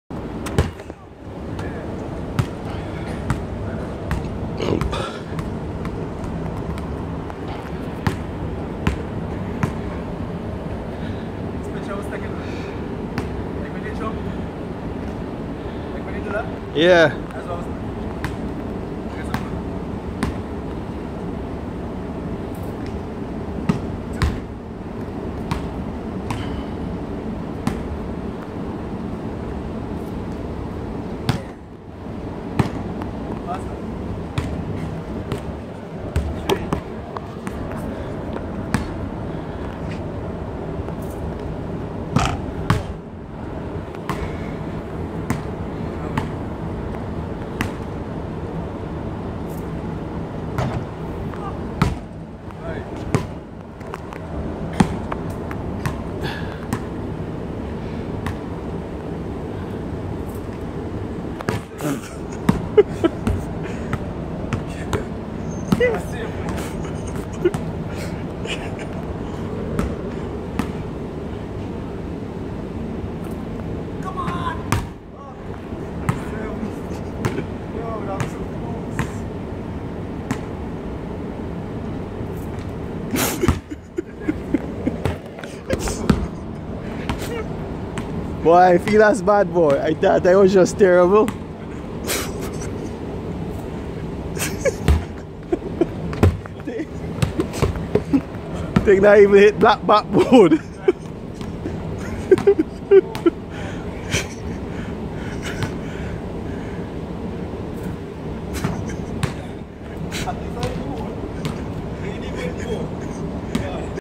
USA worst basketball 3 point shooter
World's worst basketball three point shooter tries to make a basket.